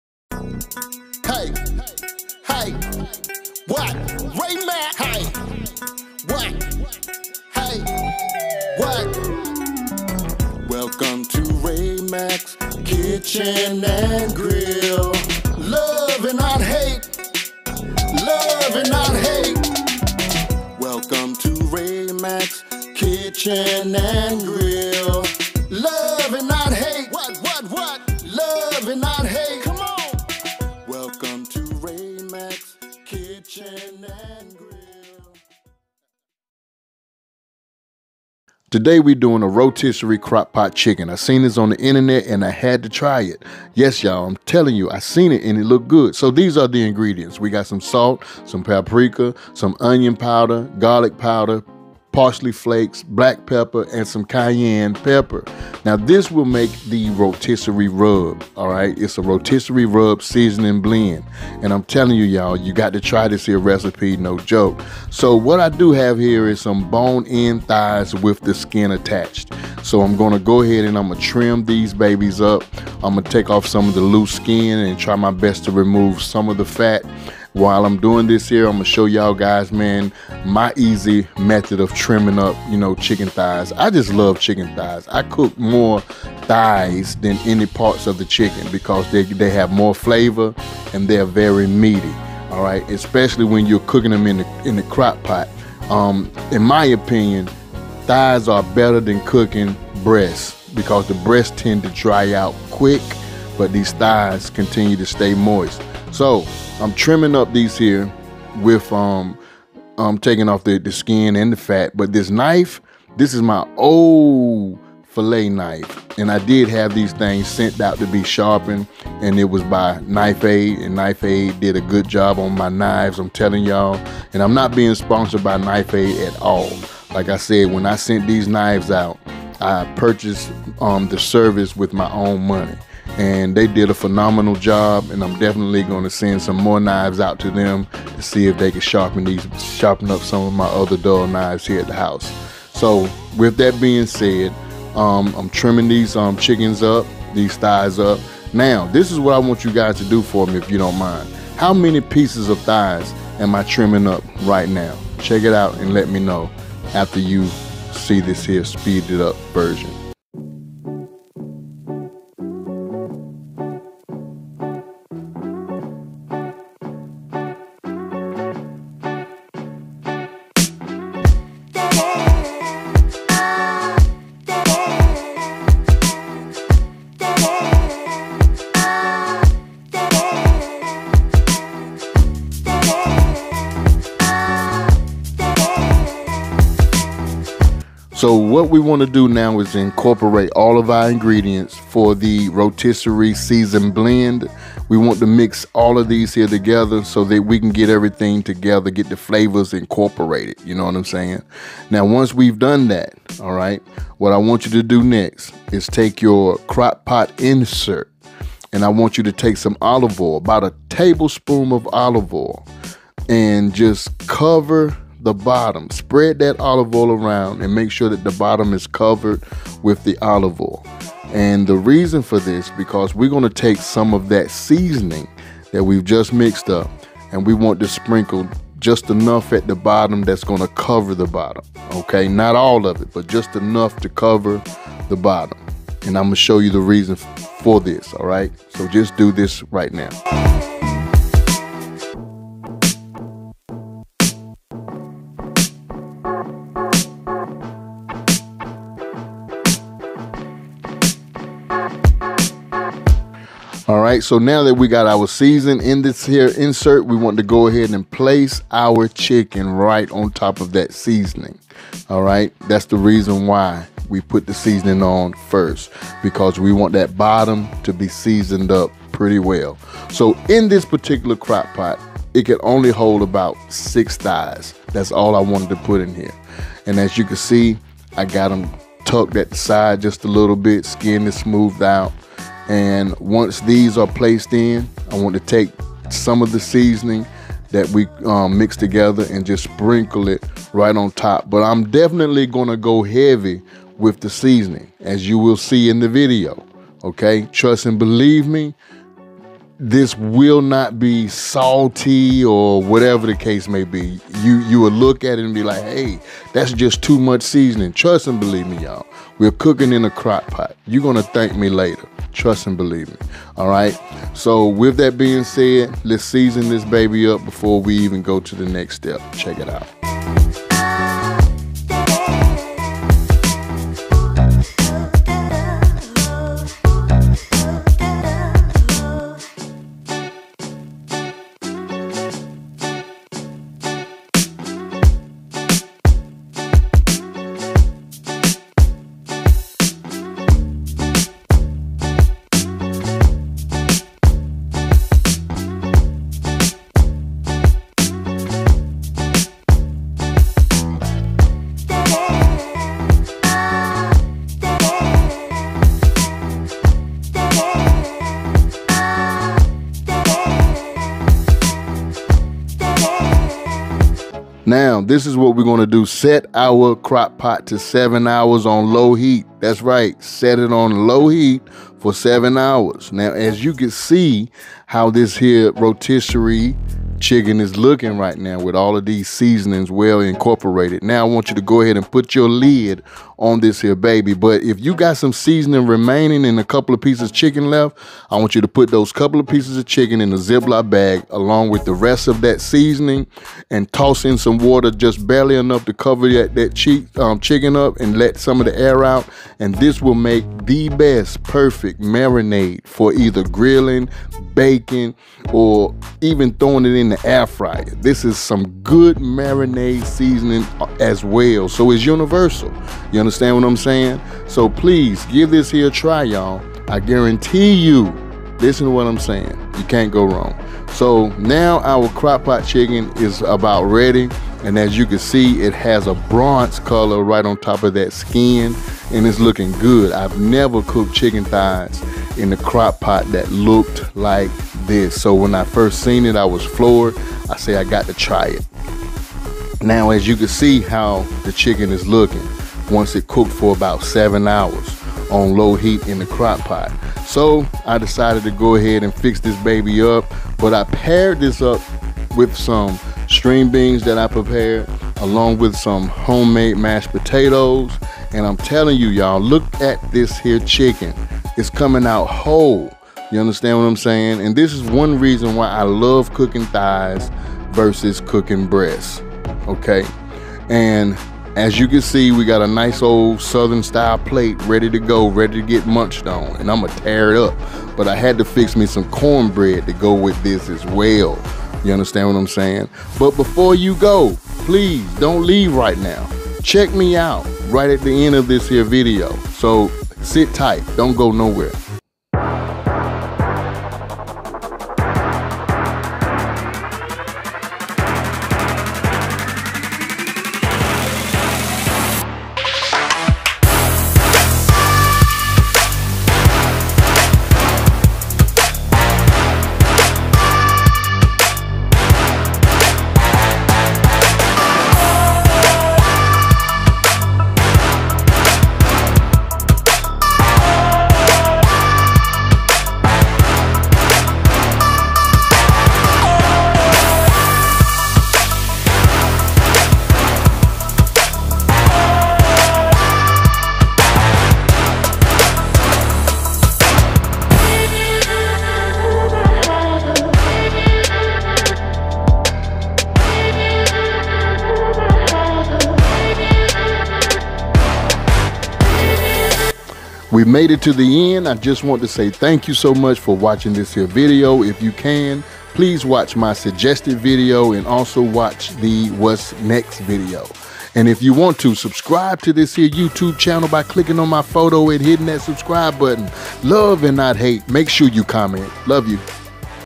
Rotisserie Crock-Pot Chicken Thighs Recipe | Crockpot Chicken Thighs
If you love soul food and southern recipes Follow me or join my Awesome Free Recipe YouTube channel for recipes, fun, tips and inspiration.😀
(CLICK HERE FOR THE RECIPE)

WHAT YOU WILL NEED:
~Family pack of chicken thigs, bone and skin on
~Rotisserie Chicken Seasoning
2 tablespoon of salt
1 tablespoon paprika
1 tablespoon onion powder
1 tablespoon dried thyme...(I had this in the middle of the seasoning)
1 1/2 teaspoon cayenne pepper
1 1/2 black pepper
1 1/2 teaspoon garlic powder

Directions:
1. Clean and pat chicken thighs dry with a paper towel, remove and trim loose skin and fat from the thighs.
2. pour olive oil on the bottom of your crockpot and spread evenly with your hand. mix your seasoning together in a bowl and sprinkle SOME of your seasoning covering the bottom of the crockpot. Place 6 or 8 thighs in your crockpot (depending on the size of your pot), and cover your chicken with the seasoning.
3. Place the lid of the chicken, set your temp. to LOW, and timer for 7 hours.
4. Serve these up with some HOMEMADE MASHED POTATOES, SOUTHERN STYLE GREEN BEANS, BAKED CORNBREAD AND A ICE COLD GLASS OF SWEET TEA.

[If you cook this recipe...Please post a picture on Instagram or Facebook using the hashtag Raymackstyle.

‐ TRY THESE SEASONINGS BELOW

Spain's Spices Instagram